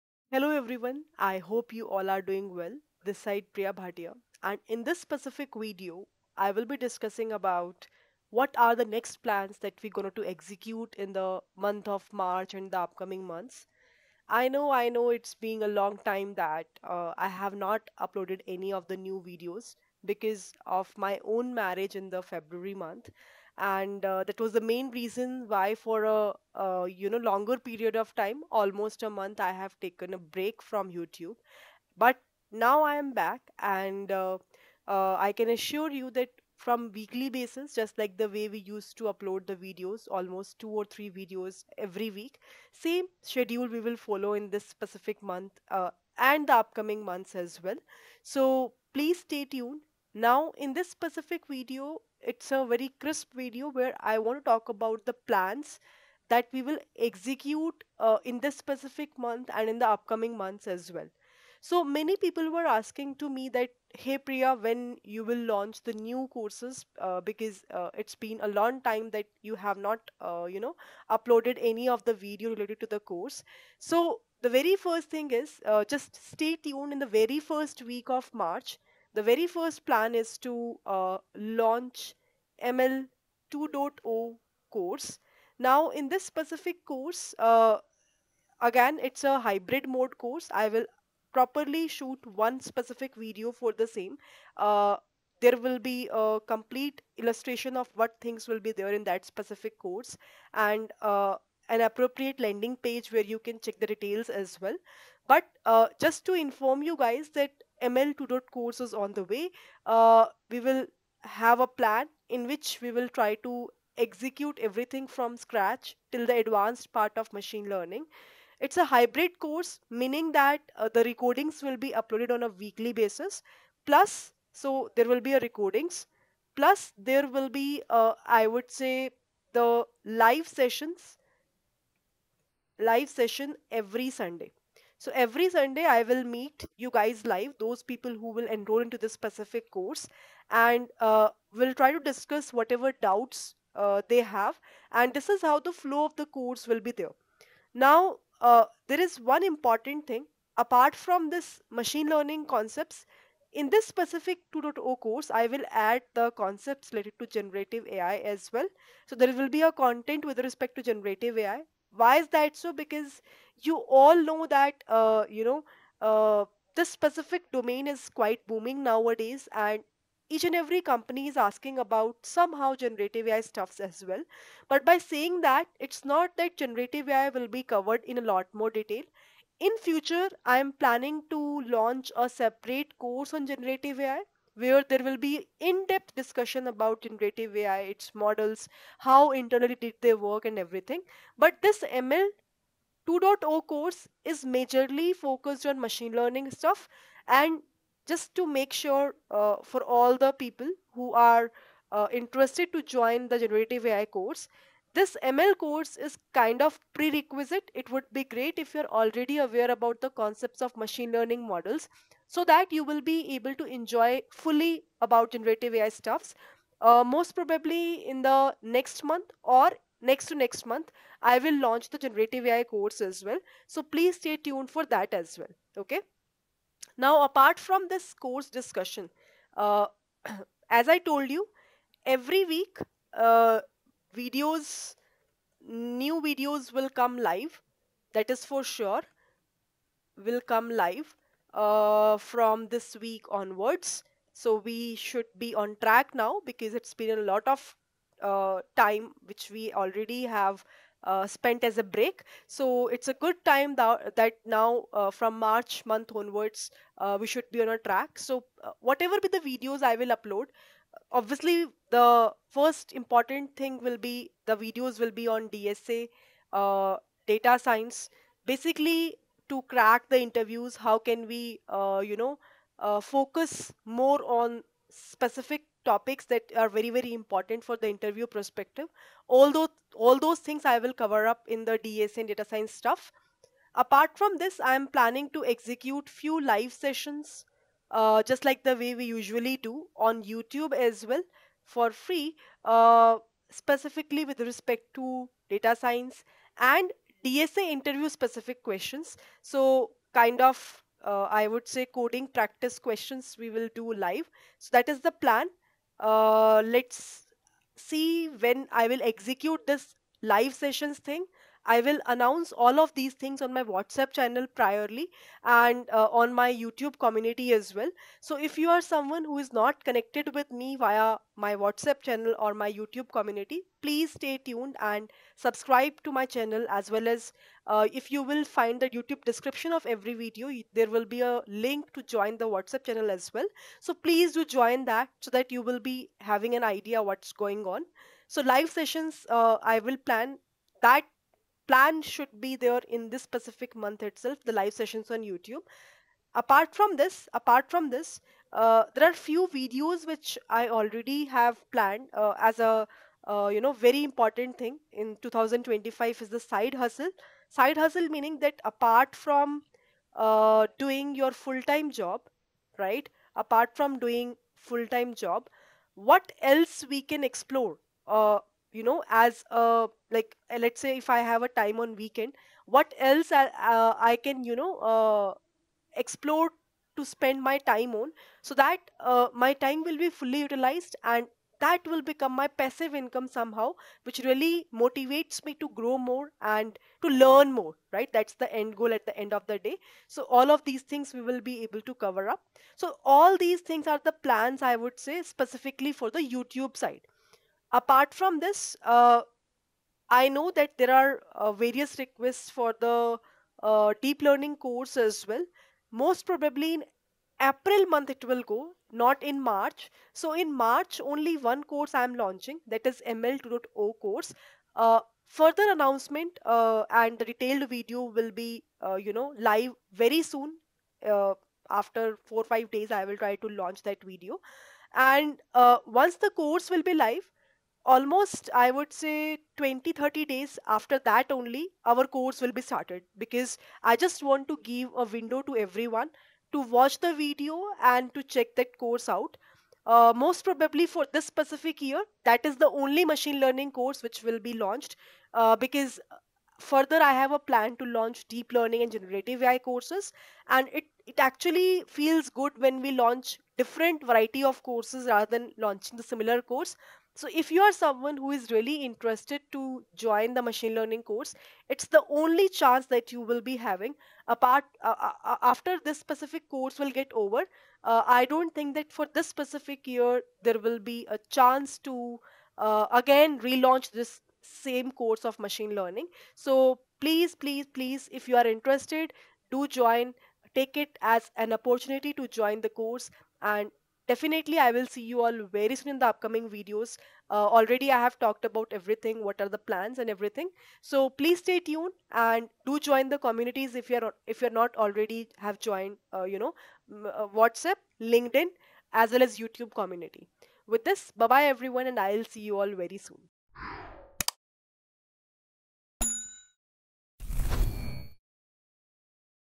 I’m Back! Big Announcements & Exciting Plans Ahead!
Hey everyone! I'm back after a month-long break, and I'm excited to share some big plans for March and beyond!